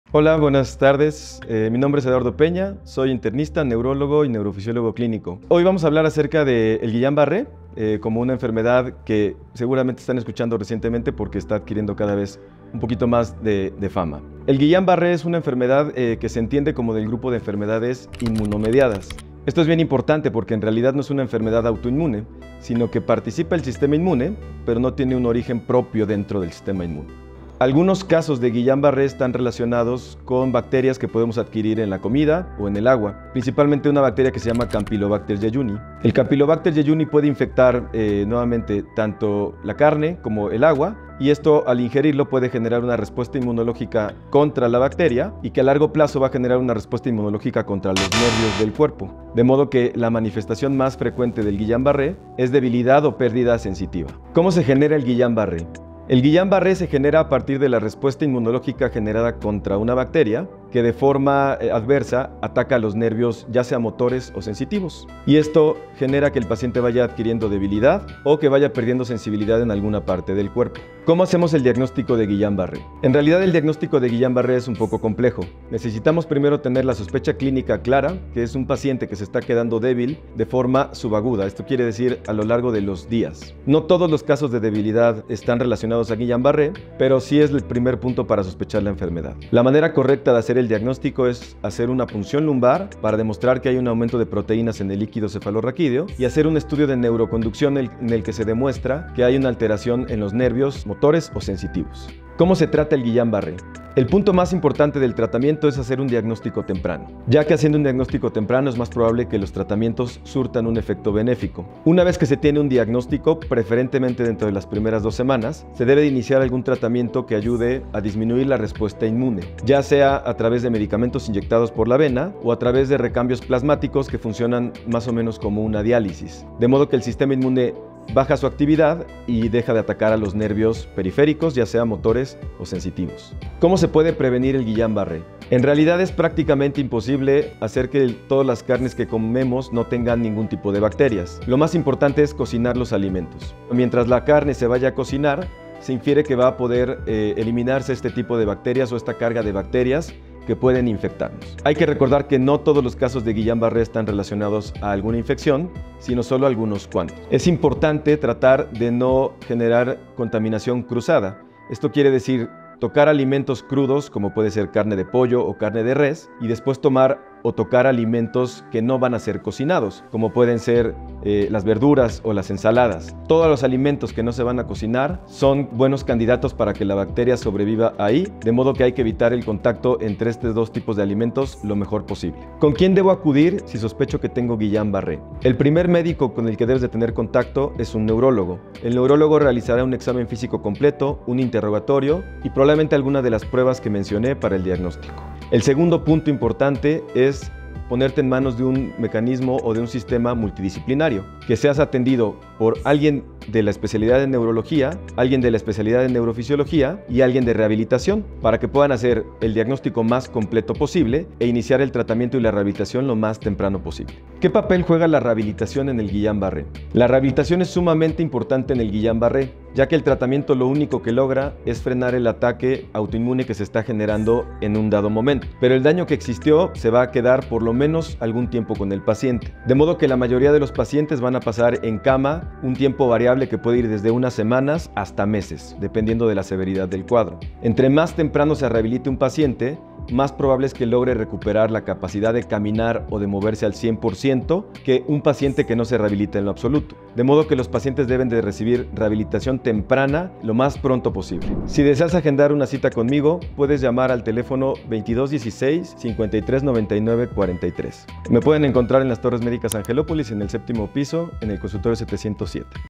📑 Guía completa del Síndrome de Guillain-Barré 🧠 | Somnoresp | Dr. Eduardo Peña
El Dr. Eduardo Peña nos platica todo acerca del Síndrome de Guillain-Barré. Desde qué es, causas, diagnóstico, tratamiento y rehabilitación.